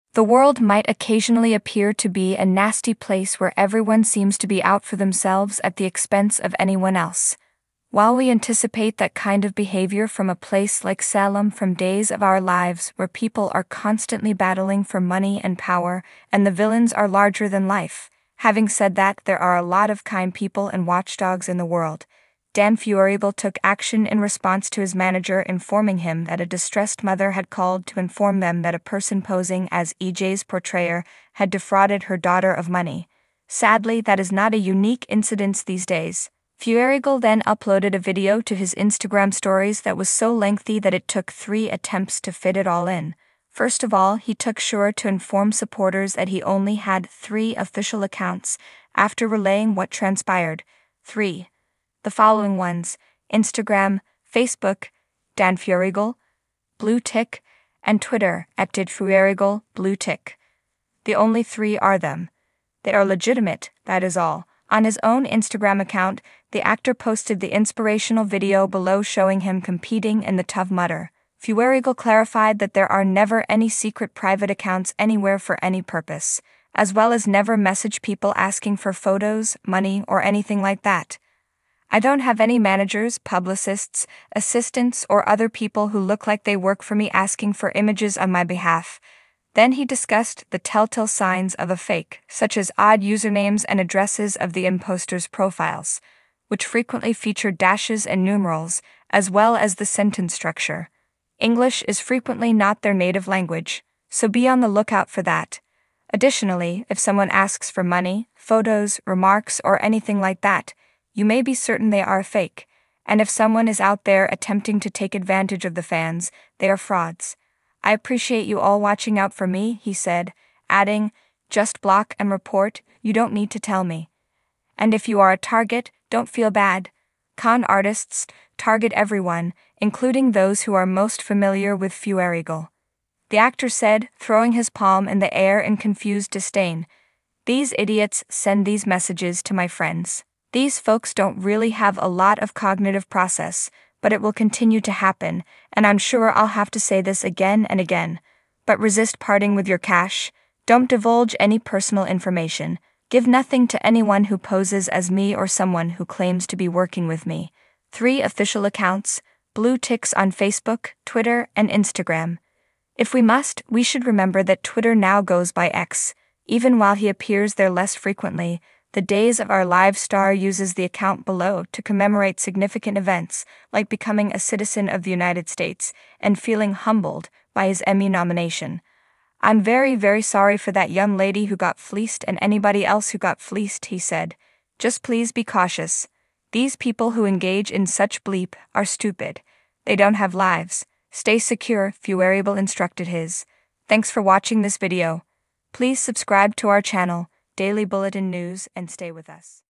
Days of Our Lives Star Drops Shocking Bombshell About Fans! You Won't Believe What He Calls Them...
From "Days of Our Lives" Dan Feuerriegel's Message to Fans Is Annoying: "These People Are Idiots"